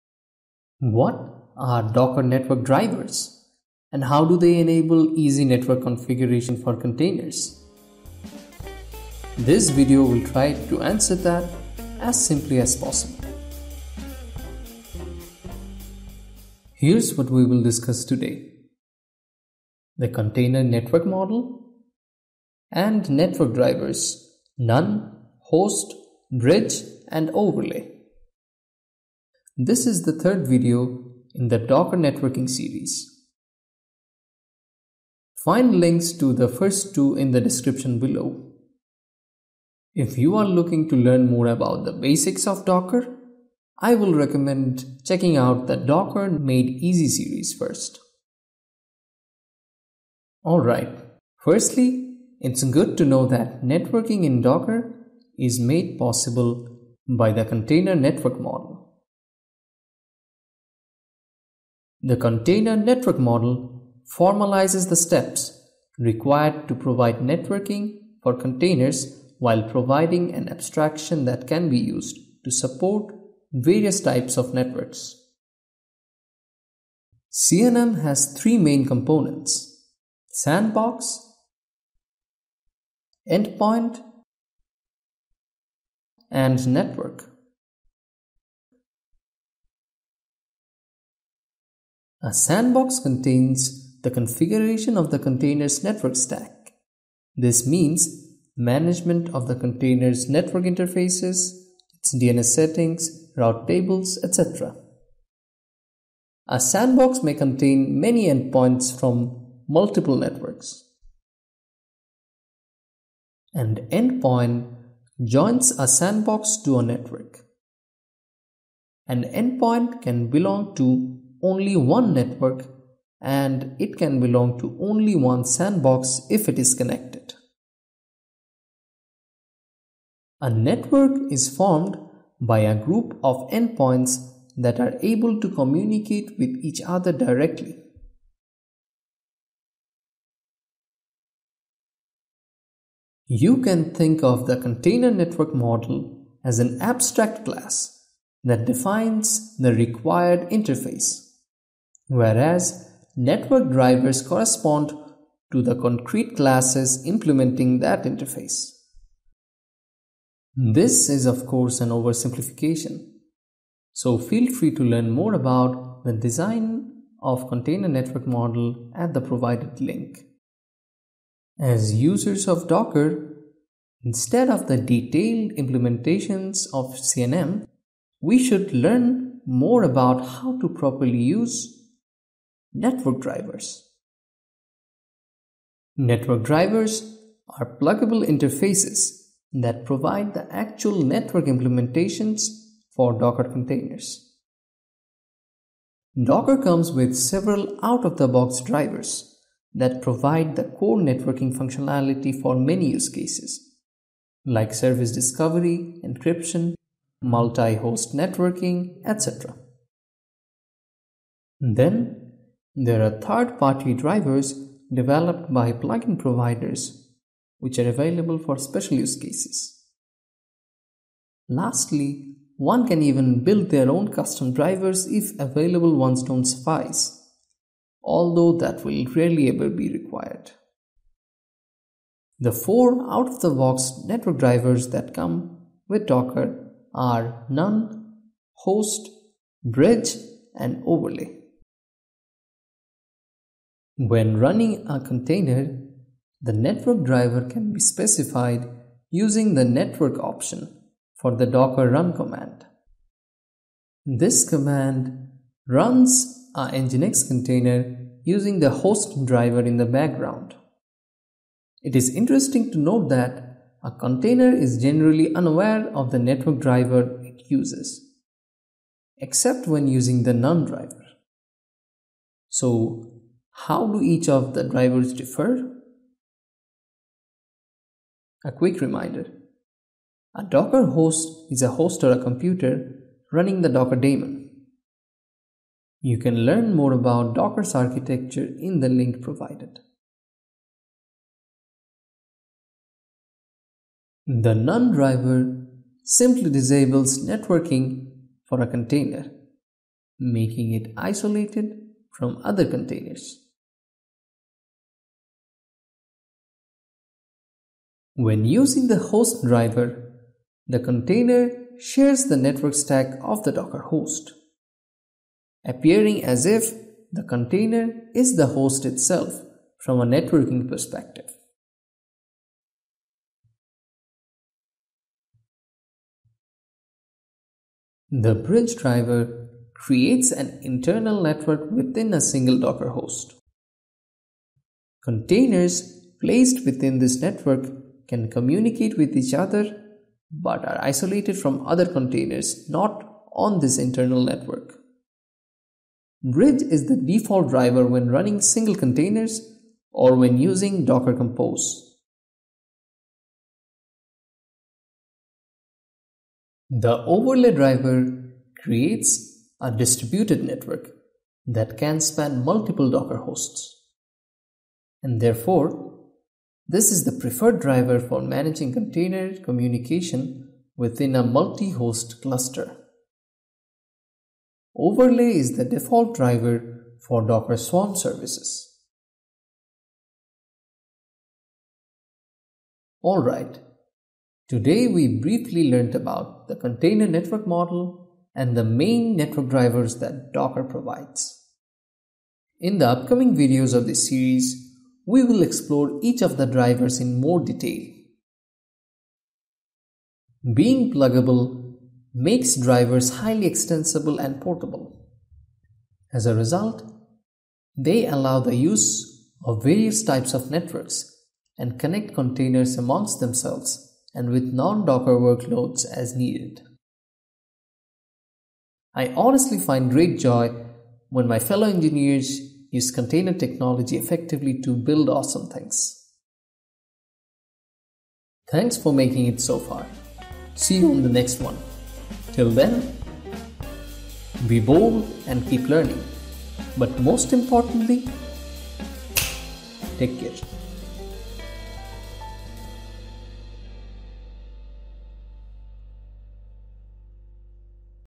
Docker Network Drivers Overview | Networking in Docker #3
Learn how Docker Network Drivers allow easy Container network configuration by implementing the Container Network Model

### REFERENCES/LINKS ###

### TIMESTAMPS ###

0:00 Intro
0:16 Agenda
0:57 Container Network Model
1:32 Sandbox
1:54 Endpoint
2:10 Network
3:03 Network Drivers
4:54 NONE driver
5:07 HOST driver
5:27 BRIDGE driver
5:56 OVERLAY driver
6:22 Conclusion

### SOCIALS ###

### CREDITS ###